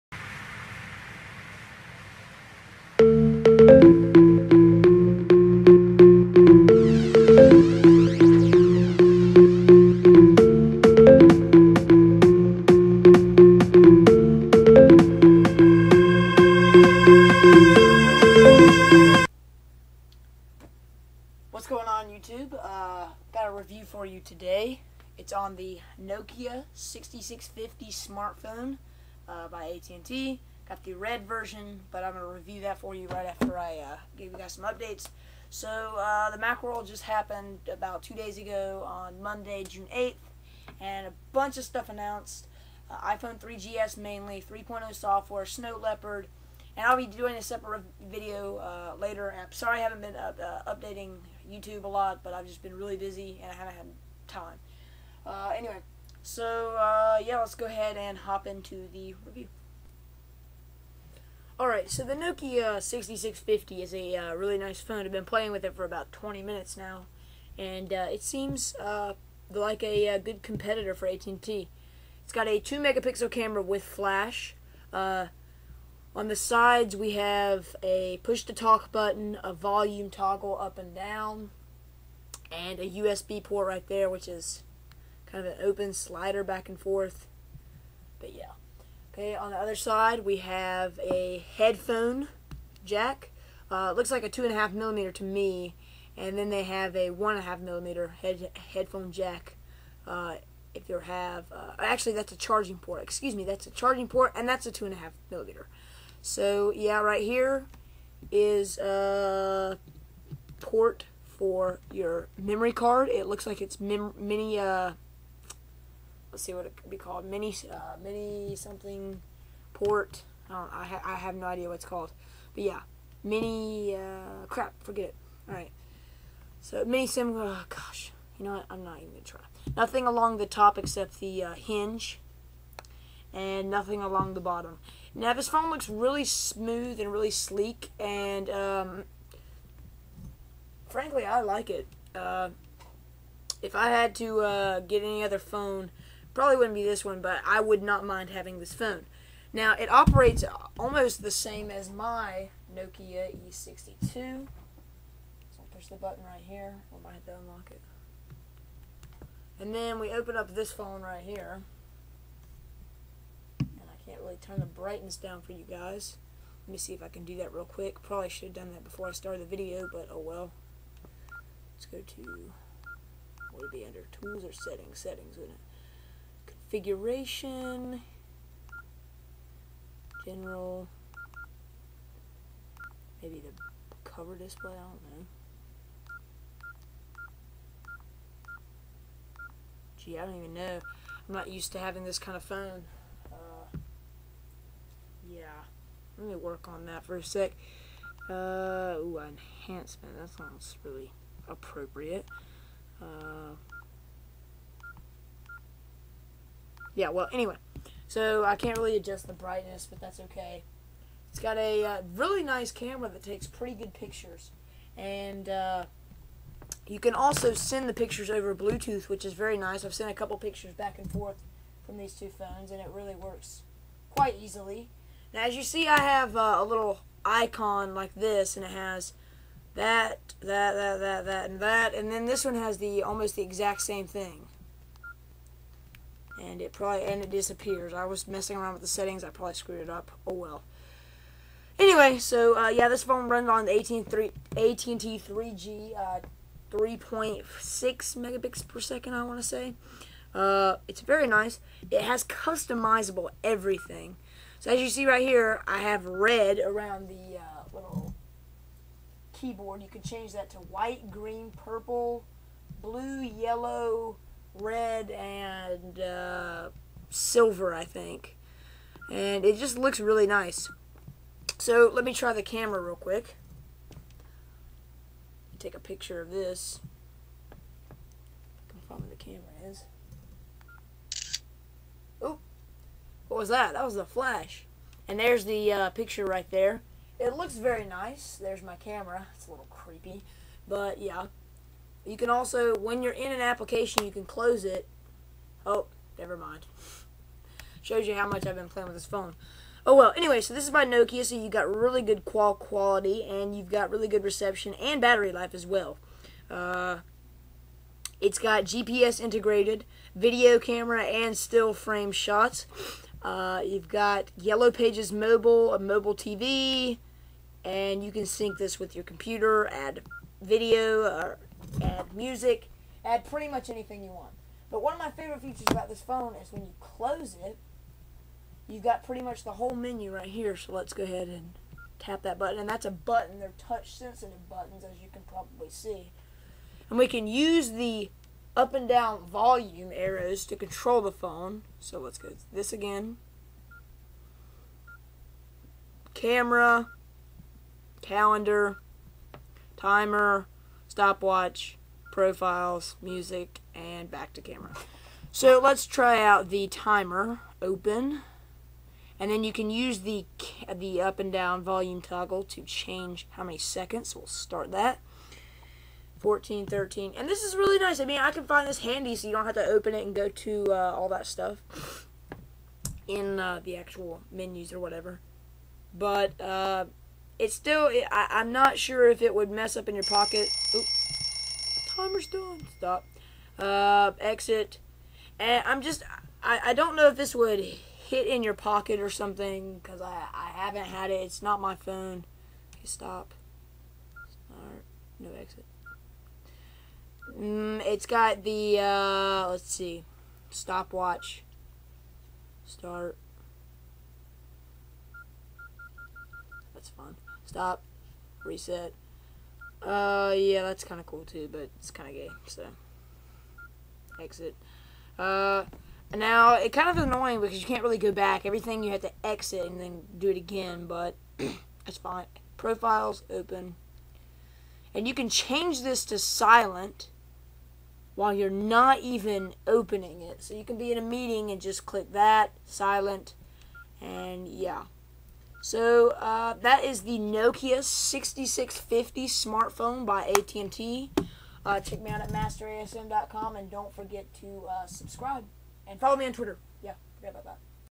Nokia 6650 SmartPhone Review
Yes this is a SmartPhone Its small lightweight and its got a great design. This phone is great for any business or regular person with a preference of clam shell design.

Pros. Bluetooth, Calender, Alarm, Gps, Yellow Pages ToGo.

Cons. No Inferred, No Tethering, and Small buttons.

8.2 Out of 10